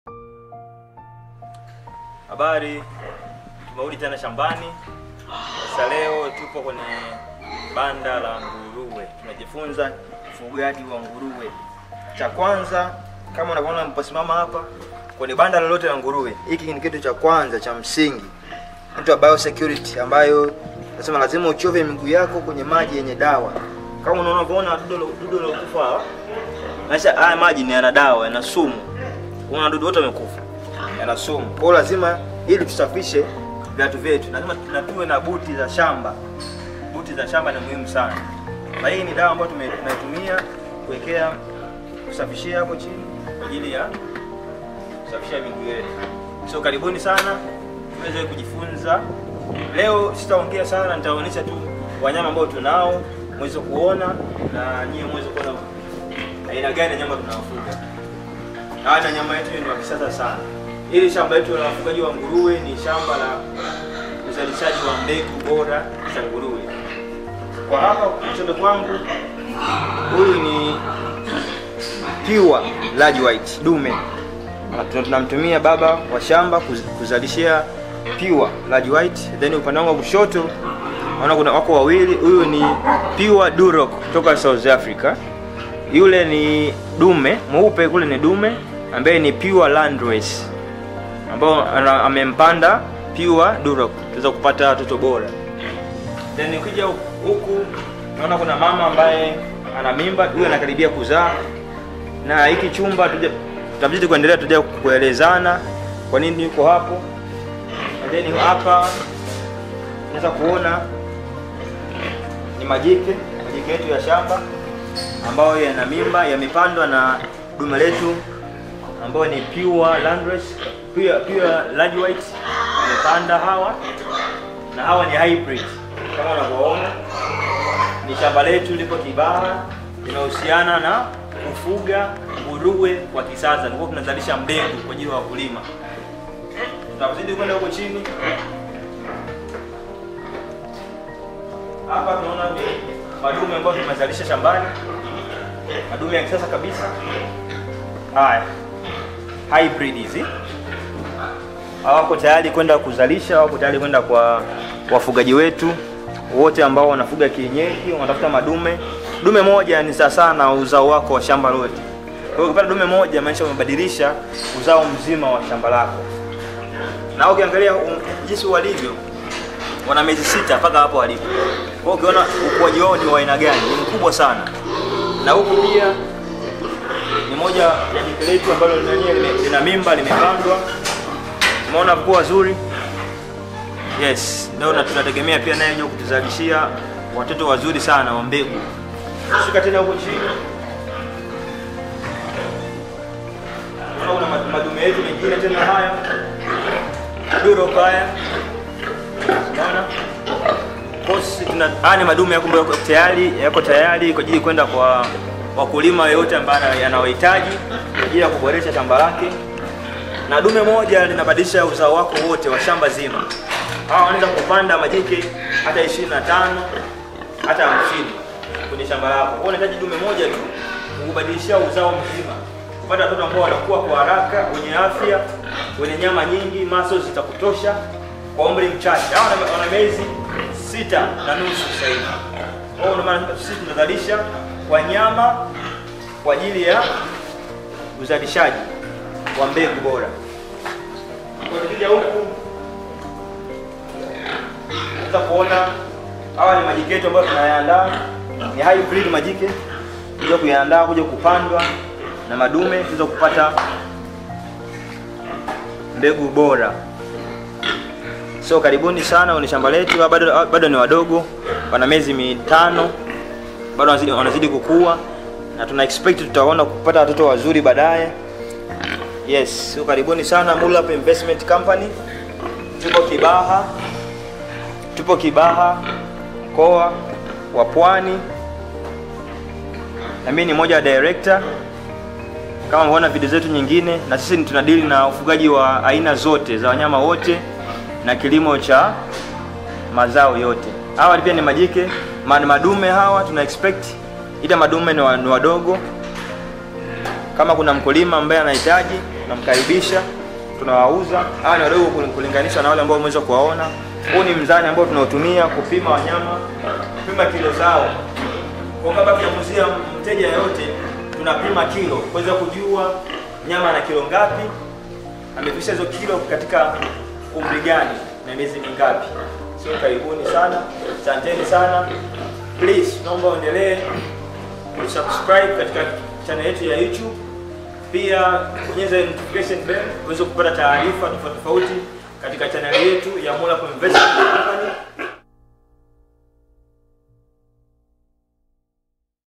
Pure and Hybrid piggery business in Tanzania(Ufugaji Nguruwe kisasa)
Highlighting about piggery at the farm and there different features and the general over view of what were all about